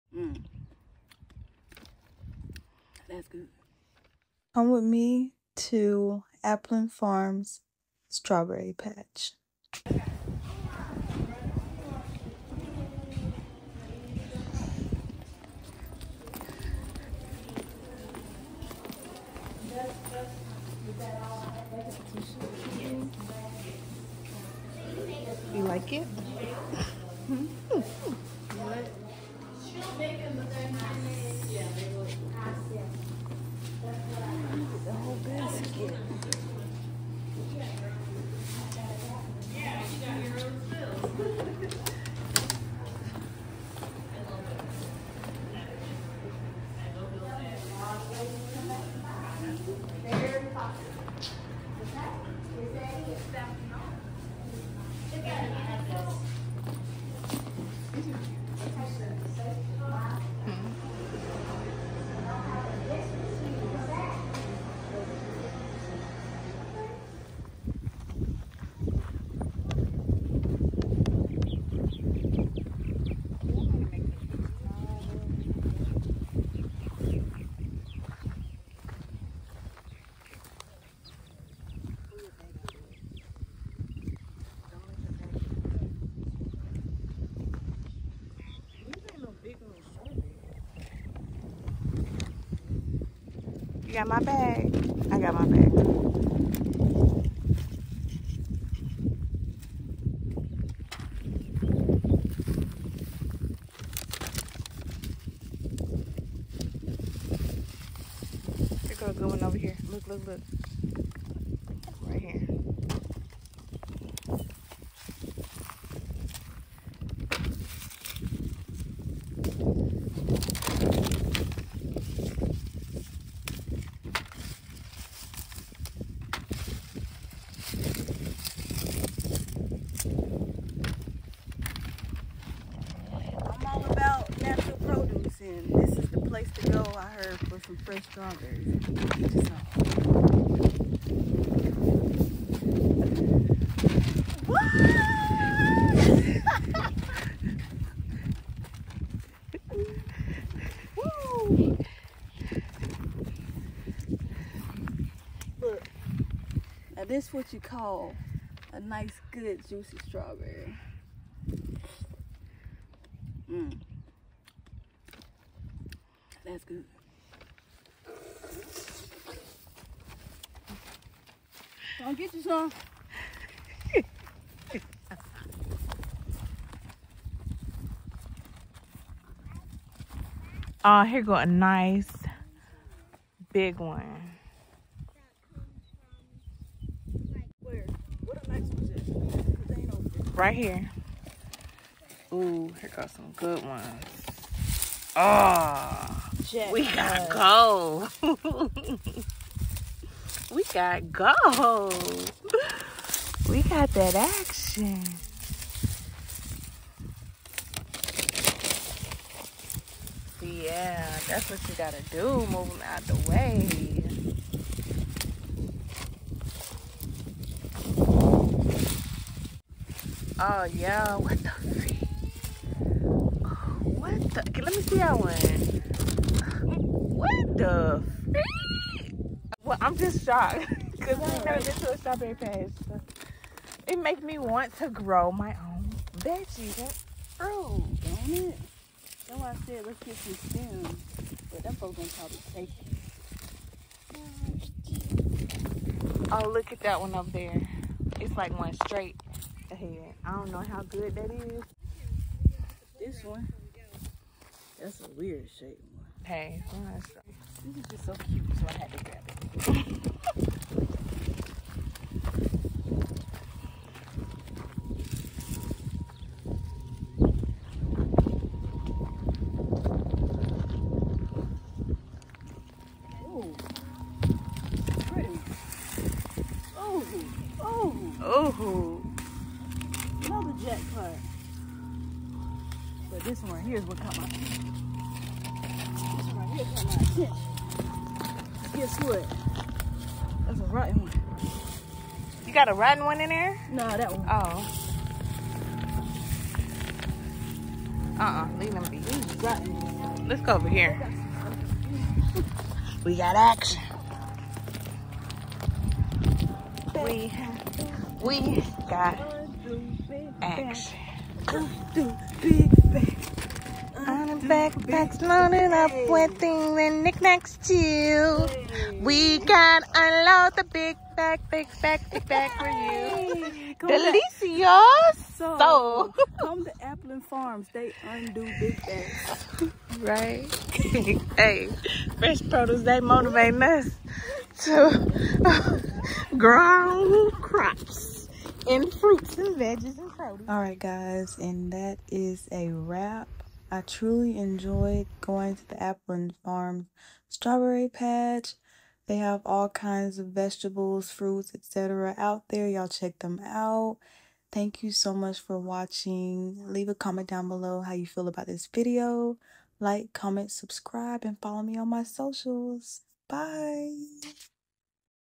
Aplin Farms| Strawberry Patch| Vlog| FRESH PRODUCE
Hey guys, Coach Salter here! This is a reminder to support your local Farms or Farmers Market! The Best place for the fresh produce. Come with me as I explore Aplin Farms!

Location: Aplin Farms  2729 N County Rd, Dothan, AL

coachsalter1
coach.tesha.t

coachsalter1